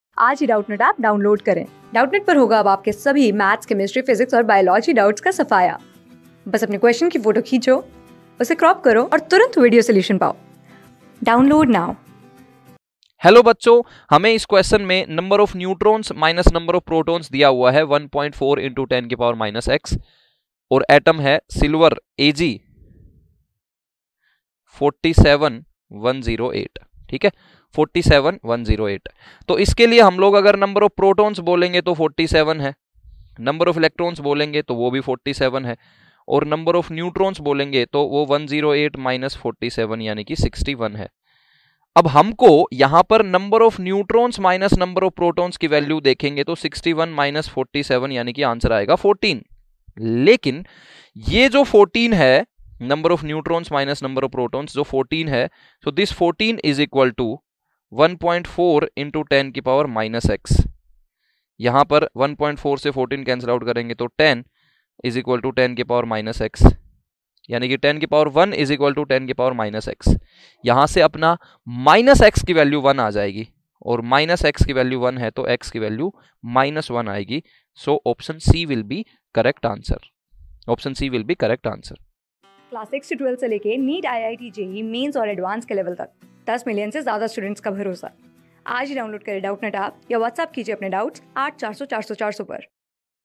For ._(47)Ag^(108), (N_n–N_p) = 1.4 × 10^(–x). Find out x | CLASS 11 | MOLE CONCEPTS | CHEMISTRY...
For ._(47)Ag^(108), (N_n–N_p) = 1.4 × 10^(–x). Find out x
Class: 11
Subject: CHEMISTRY
Chapter: MOLE CONCEPTS
Board:INTERNAL

👉 Have Any Query? Ask Us.